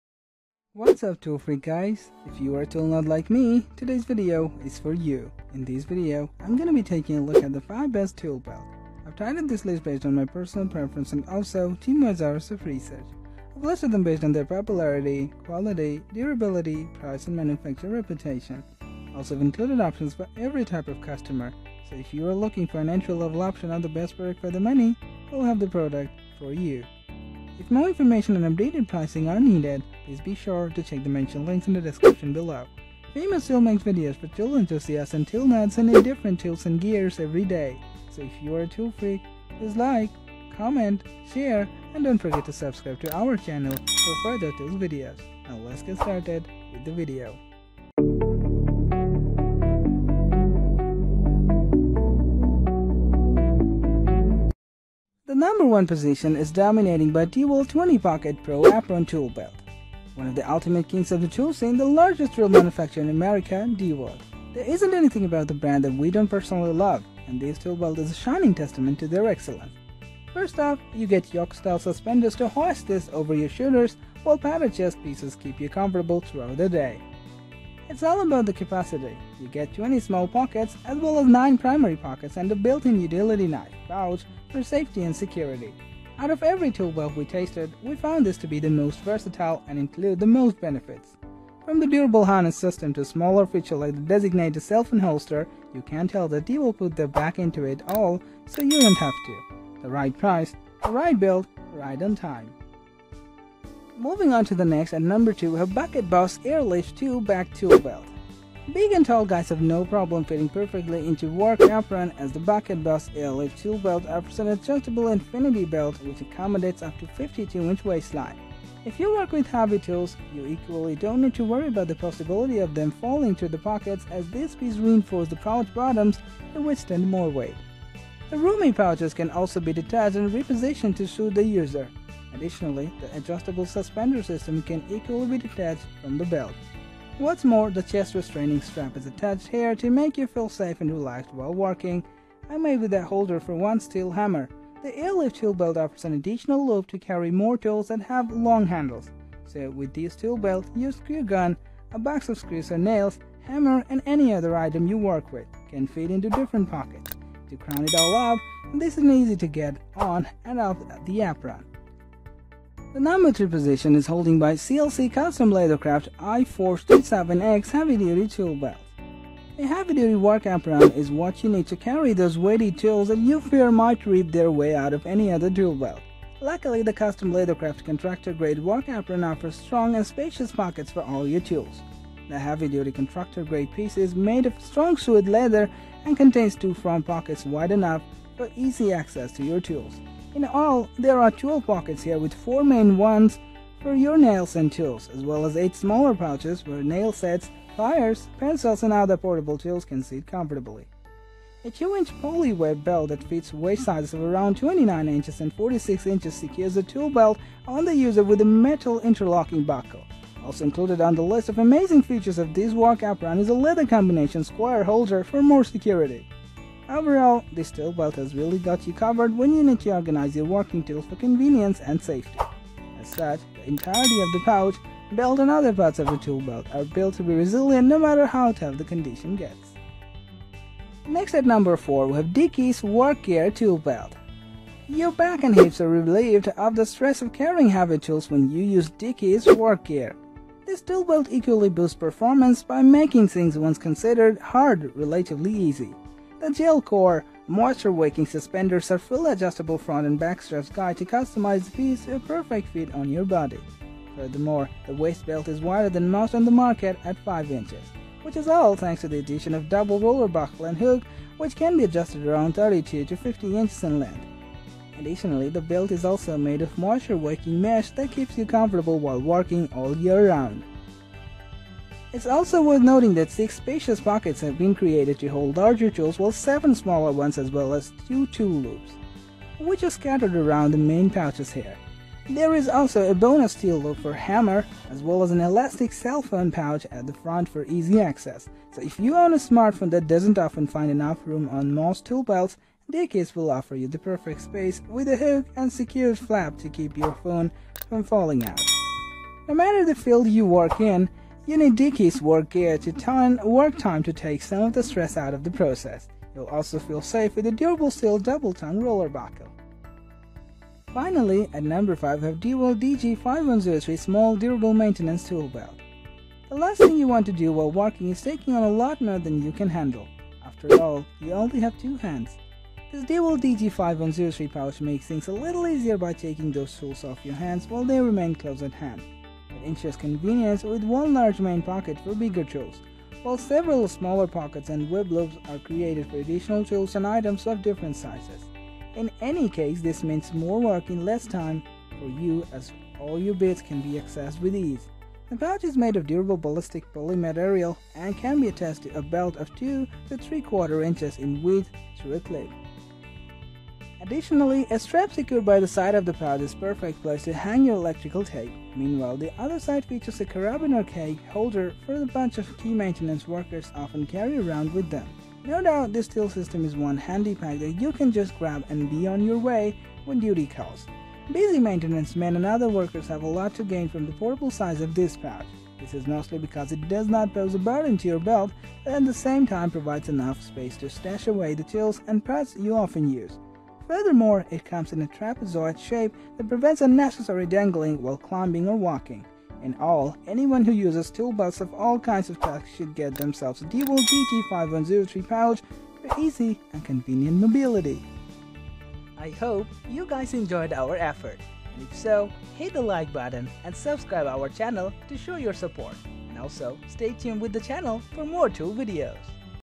Best Tool Belt In 2024 - Top 5 Tool Belts Review
Links to the best Tool Belts we listed in today's Tool Belt review video:

1. DEWALT DG5617  Tool Belt:

2. Bucket Boss - AirLift Tool Belt:

3. CLC Custom Leathercraft I427X Tool Belt:

4. Dickies 4-Piece Carpenter Tool Belt:

5. DEWALT DG5103 Tool Belt:

What are the Best Tool Belts in 2024?

In today's video we reviewed the top 5 best Tool Belts on the market in 2024. We made this list based on our personal opinion and we ranked them in no particular order, after doing our research based on their prices, quality, durability, brand reputation and many more.

After watching this video you will know which are the best budget, top selling and top rated Tool Belts on the market today.

If you choose from this list you can be sure that you buy one of the best Tool Belt available today.